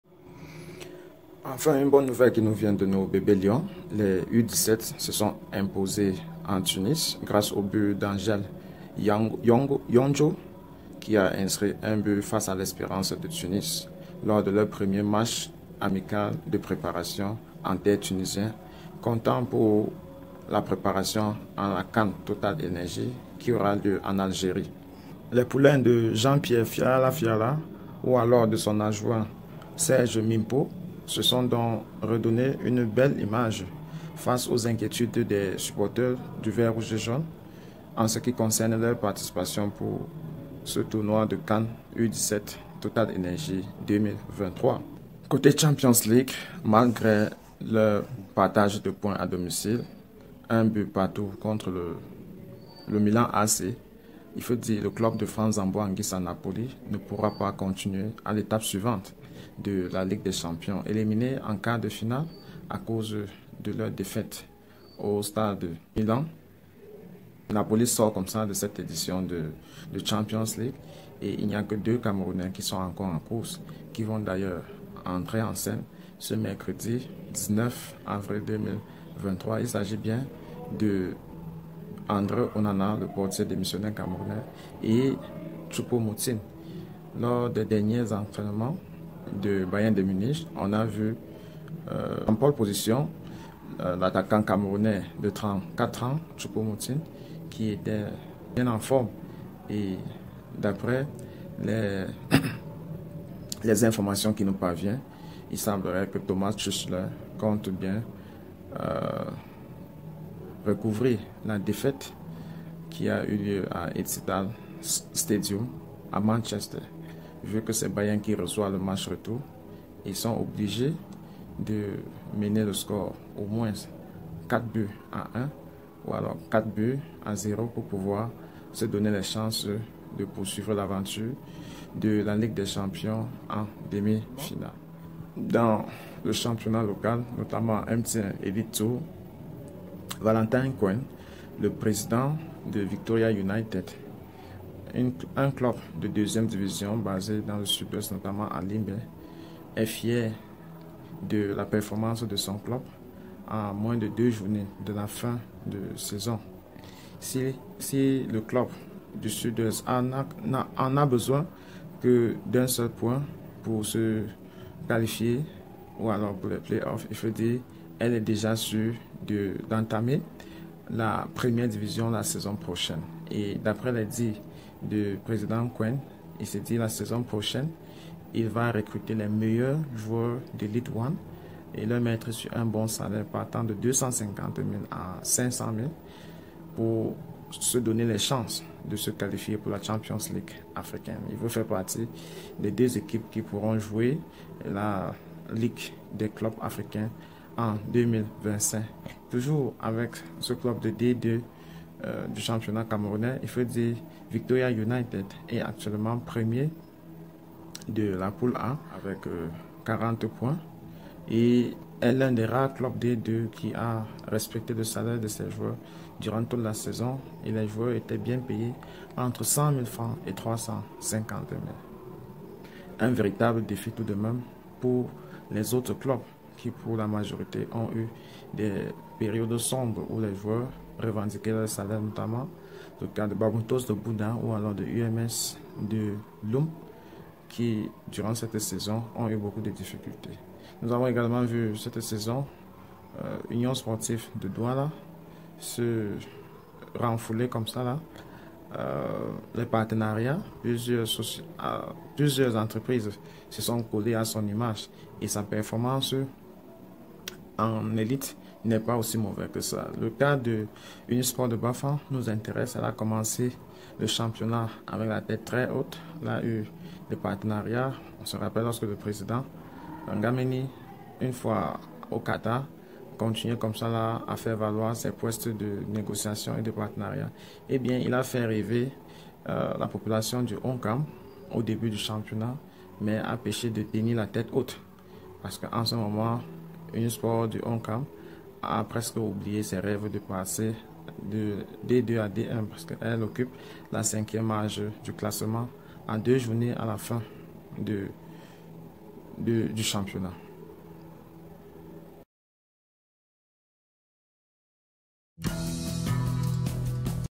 Cameroun Football : Les U17 vers un nouveau sacre | Choupo Moting surprise de UCL
En battre l'espérance de Tunis 0:1 les bébé lions renouvel ses sentiments avec les fans du ballon rond